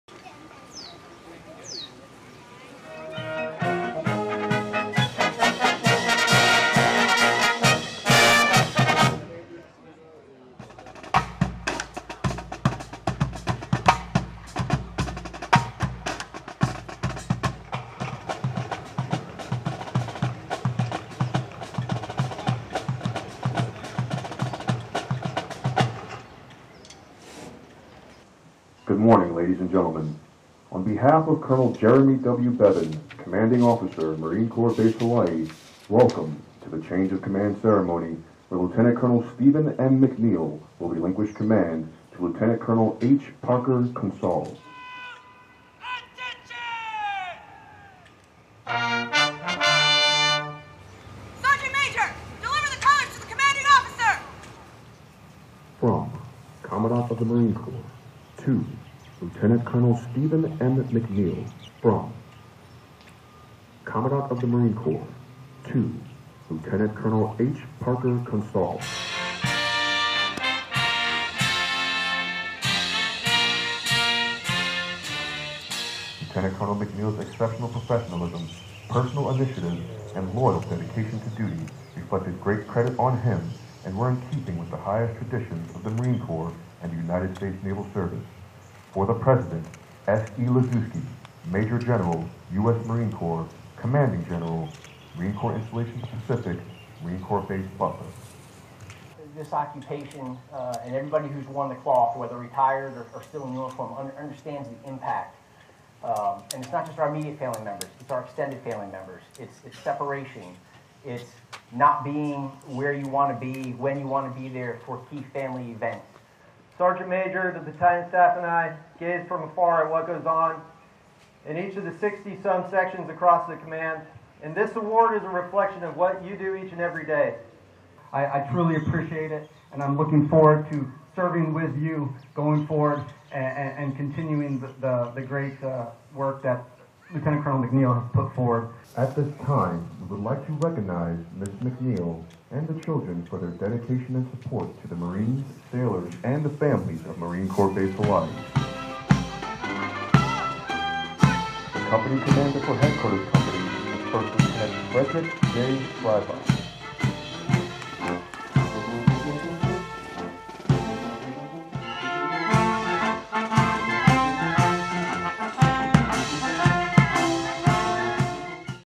Headquarters Battalion, MCBH Change of Command Ceremony
U.S. Marines with Headquarters Battalion, Marine Corps Base Hawaii, conduct a change of command ceremony at MCBH, June 23, 2023.

MARINE CORPS BASE HAWAII, HI, UNITED STATES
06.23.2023

 Lt. Col. Stephen M. McNeil relinquished command to Lt. Col. Harry P. Consaul. (U.S. Marine Corps video by Cpl. Brandon Aultman)

Film Credits: Video by Cpl. Brandon Aultman
Marine Corps Base Hawaii